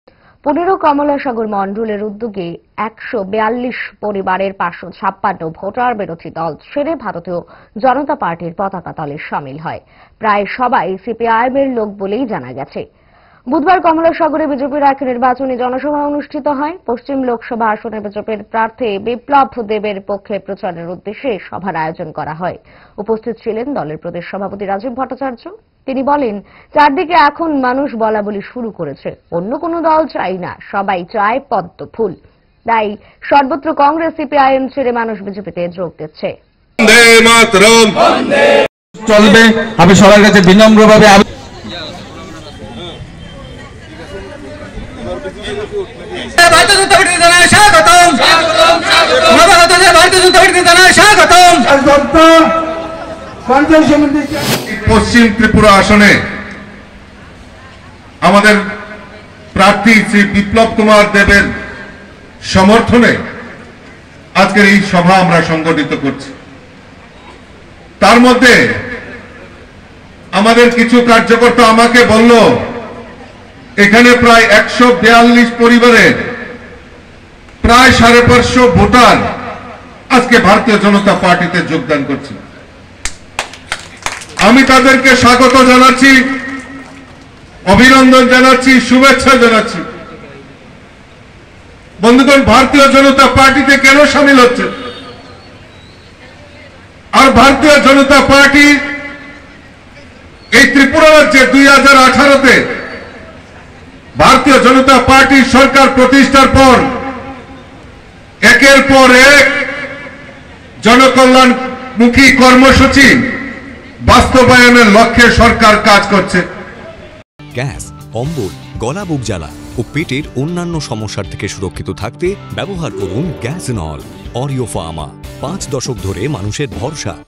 ১৪২ পরিবারের ৫৫৬ জন ভোটারের বিজেপিতে যোগ।
কমলাসাগরে ১৪২ পরিবারের ৫৫৬ জন ভোটার বিরোধী দল ছেড়ে যোগ দিলো ভারতীয় জনতা পার্টিতে। নবাগতদের দলীয় পতাকা হাতে দিয়ে দলে বরণ করে নিলেন দলের প্রদেশ সভাপতি রাজিব ভট্টাচার্য।